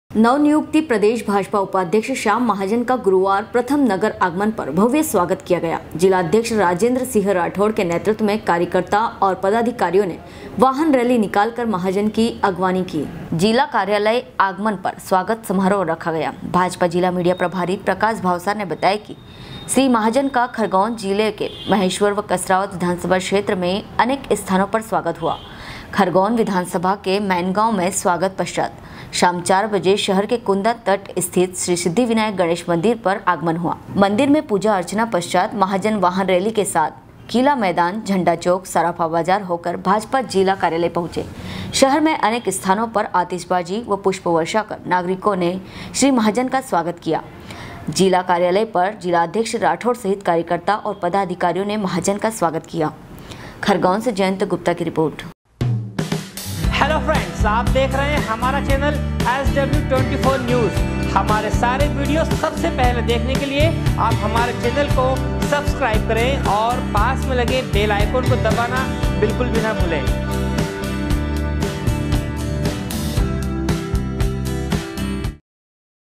SW 24 News Madhyapradesh
नवनियुक्त प्रदेश भाजपा उपाध्यक्ष श्याम महाजन का गुरुवार प्रथम नगर आगमन पर भव्य स्वागत किया गया। जिलाध्यक्ष राजेंद्रसिंह राठौर के नेतृत्व में कार्यकर्ता और पदाधिकारियों ने वाहन रैली निकालकर महाजन की अगवानी की। खरगोन से जयंत गुप्ता की रिपोर्ट